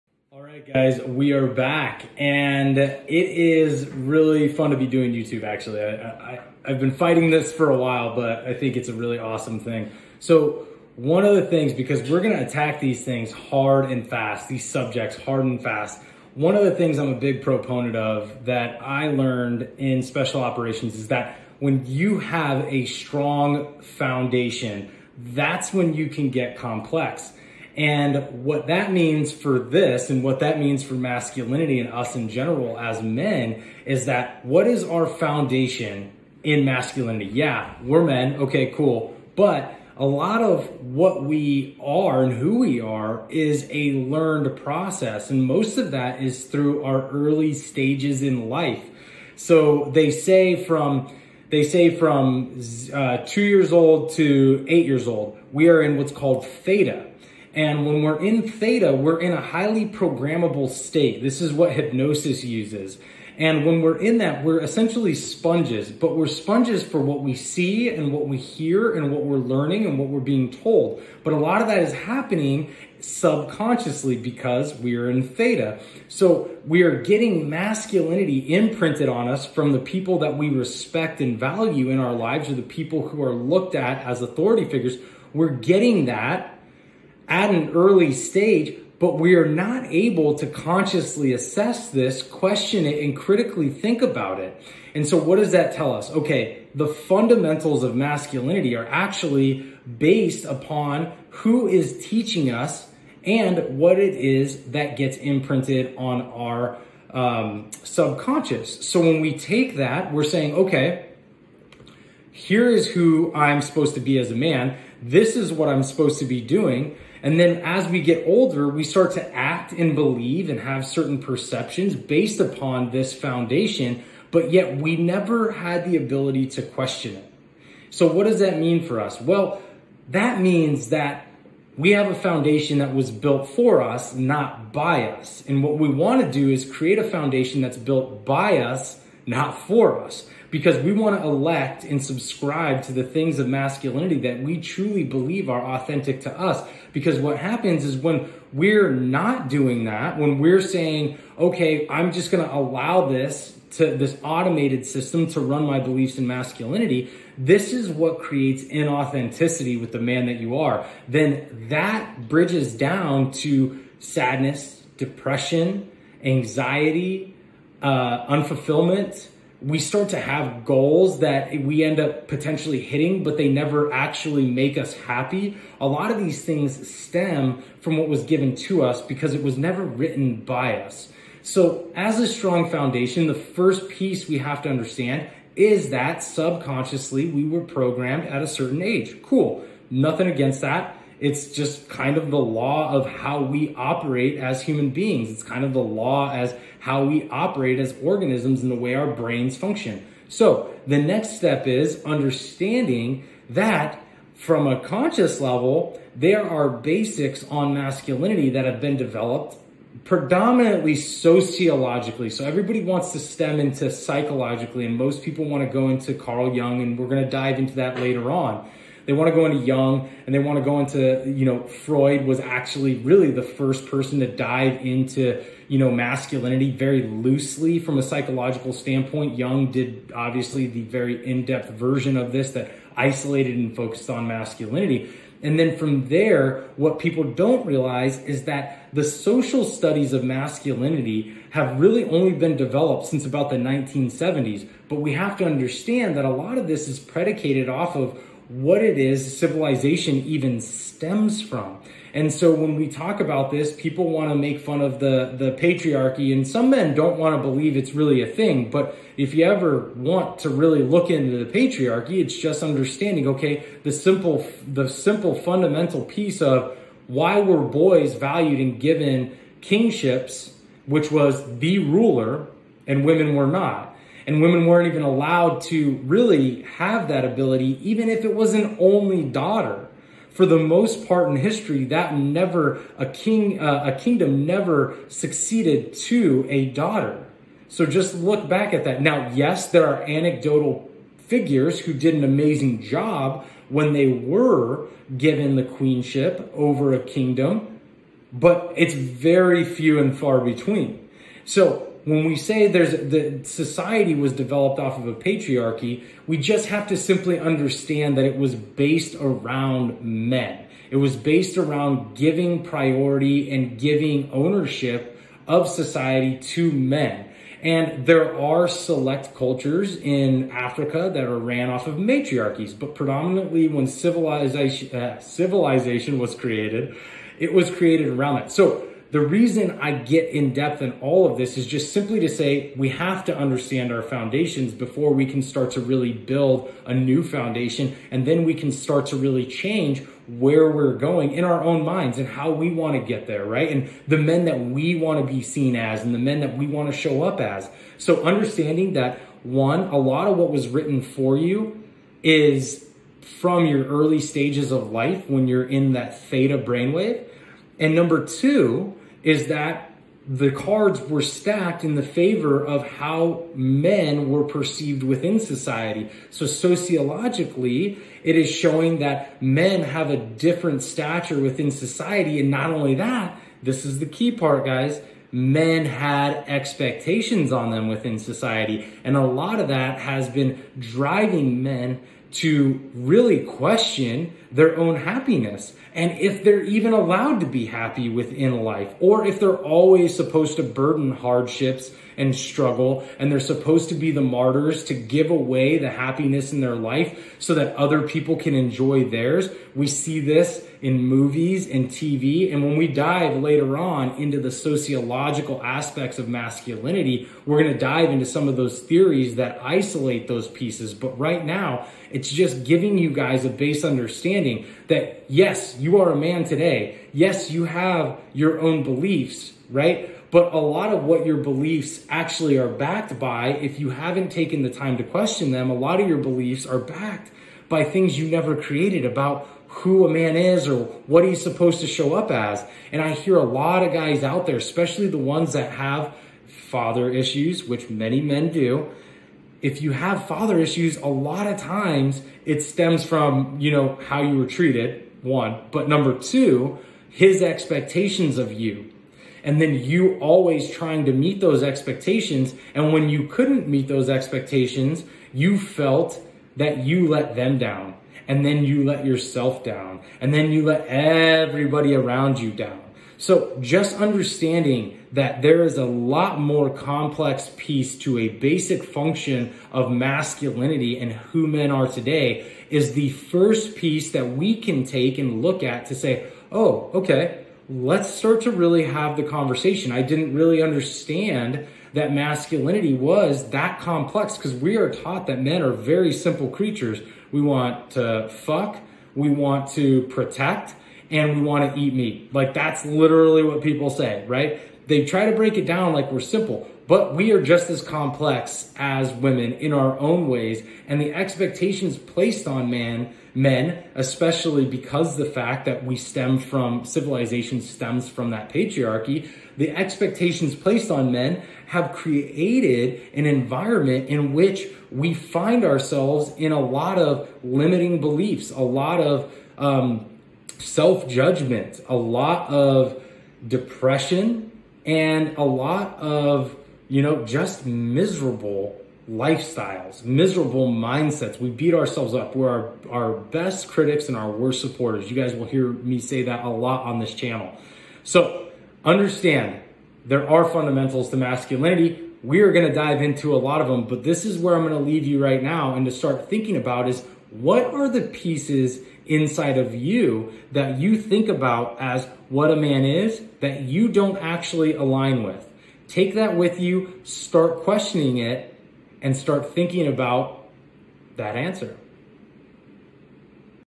The Foundations of Masculinity
Men have expectations on them within society, which drives men to question their own happiness…and more importantly are we even allowed to be happy in real life or must we always struggle with self-judgement, depression, and living with critical mindsets.

A good place to start is by looking inwards and seeing: What are the pieces inside of you that you’ve been programmed to think are ‘what it means to be a man’ but that actually don’t align with you?
Start questioning this. This is the work. You got this brother.

If you'd like to hear more, check out The Art of Masculinity podcast